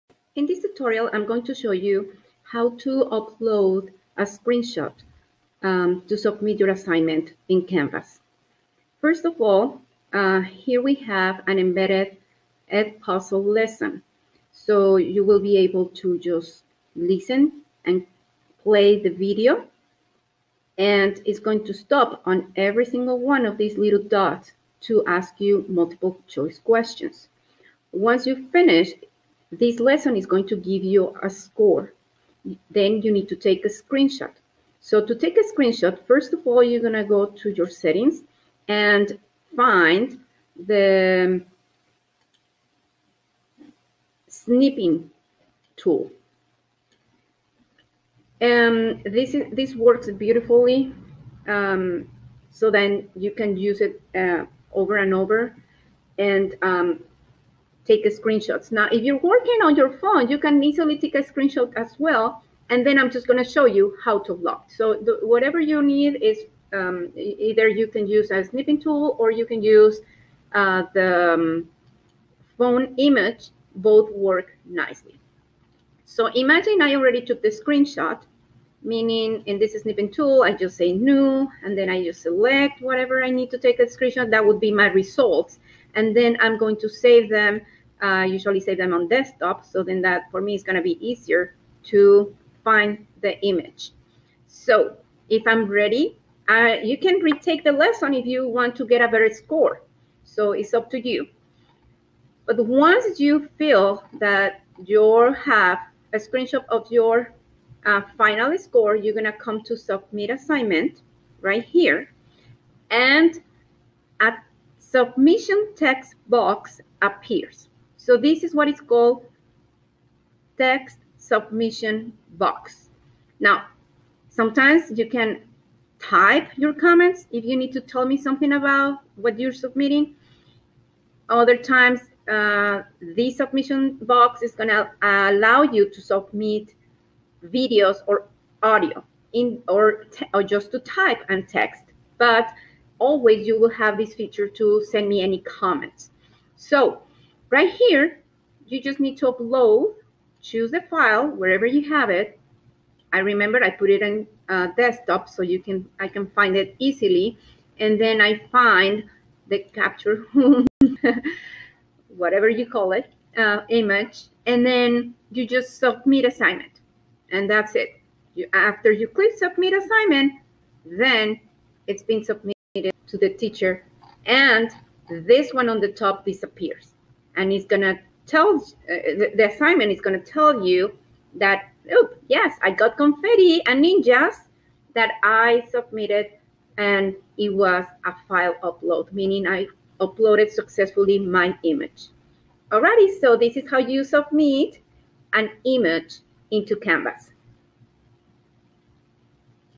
How to complete an embedded edpuzzle activity, and submit a screenshot of the results in CanvasLMS
This is a tutorial on how to submit assignments that require the student to submit an image or a screenshot. The student needs to FIRST click the SUBMIT button for the submission BOX to appear where the student will upload the image.